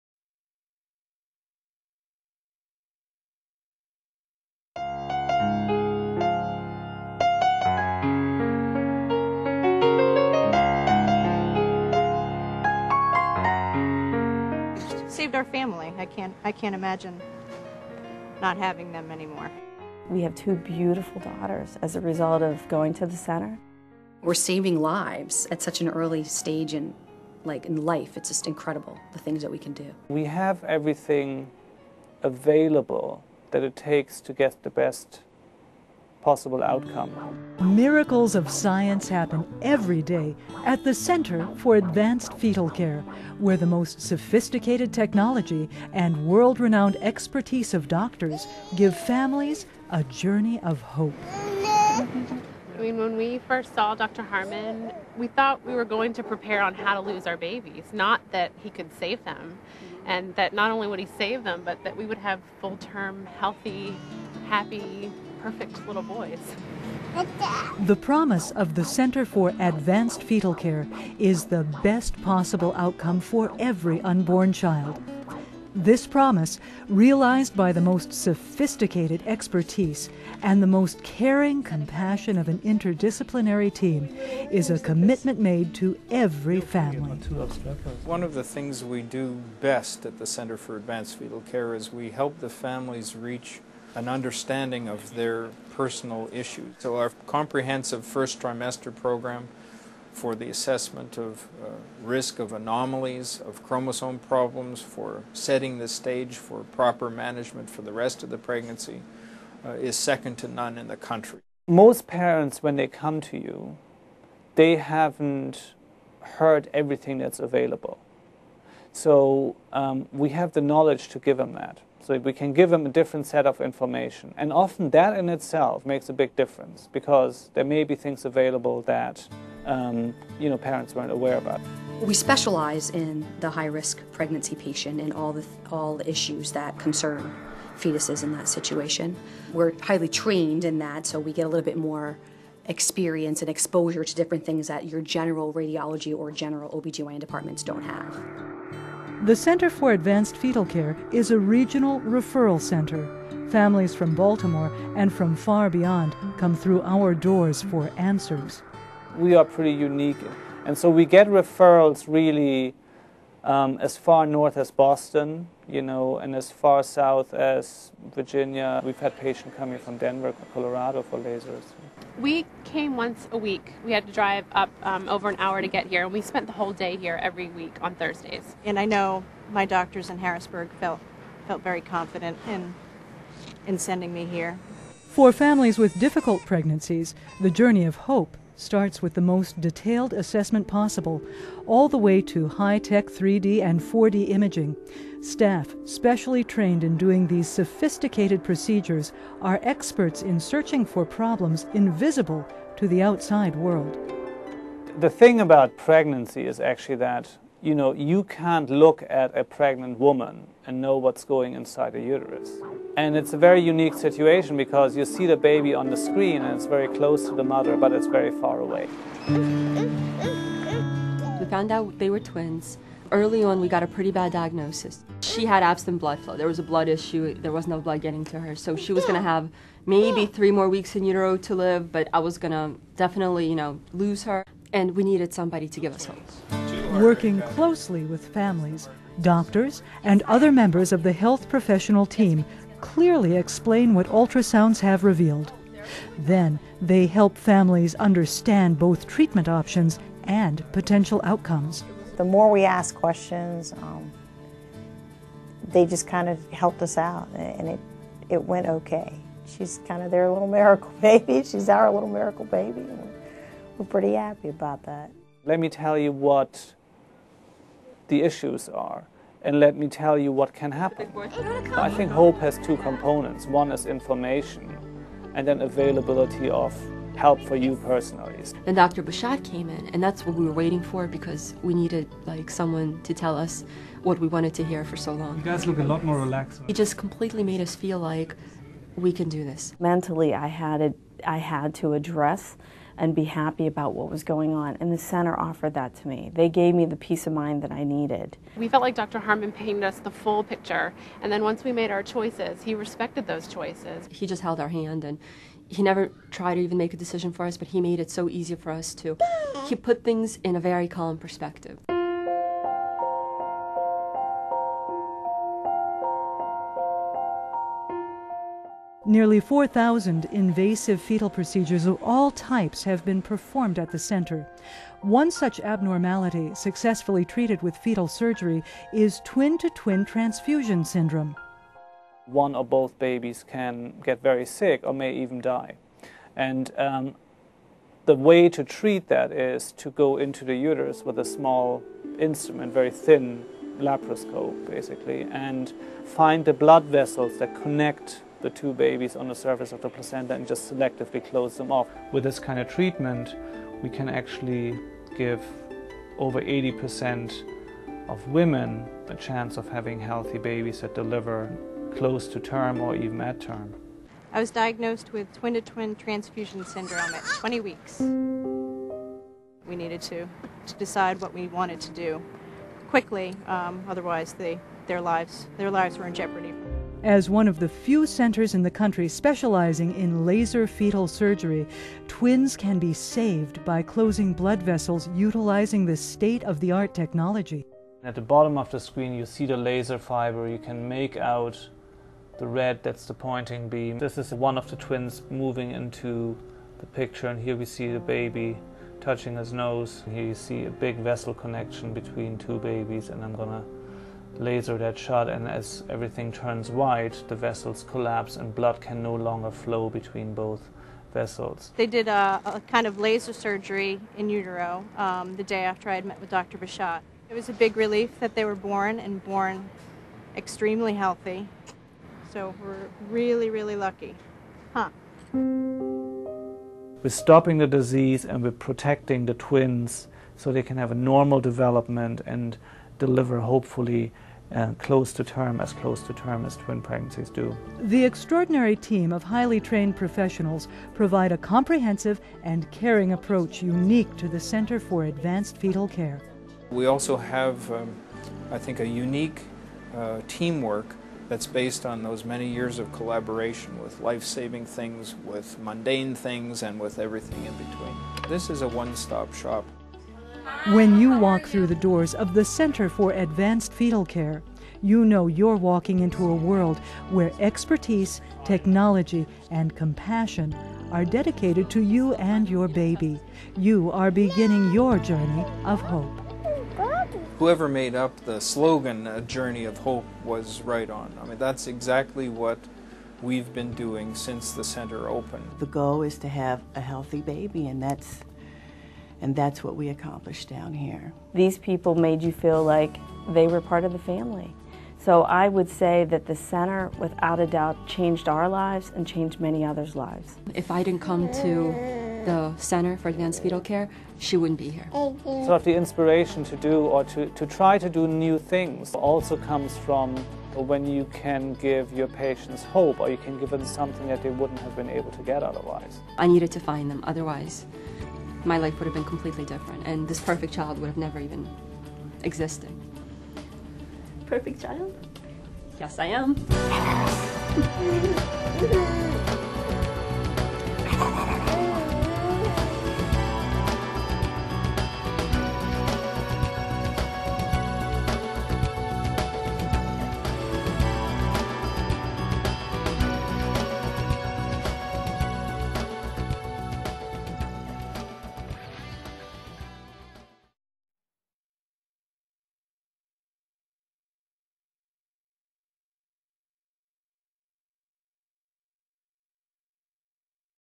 The University of Maryland Center for Advanced Fetal Care
The University of Maryland Center for Advanced Fetal Care is a world-class clinical and research facility dedicated to the care of the fetal patient and the unborn baby's family. The Center has rapidly gained a reputation as the premier resource in the region for state-of-the-art perinatology -- the care and support of the unborn child. Physicians of the Univeristy of Maryland School of Medicine Department of
Obstetrics, Gynecology and Reproductive Sciences have performed more than 3,000 successful intrauterine fetal procedures, among them hundreds of life-saving fetal transfusions, drainage procedures, intrauterine shunt catheters and other fetal surgery.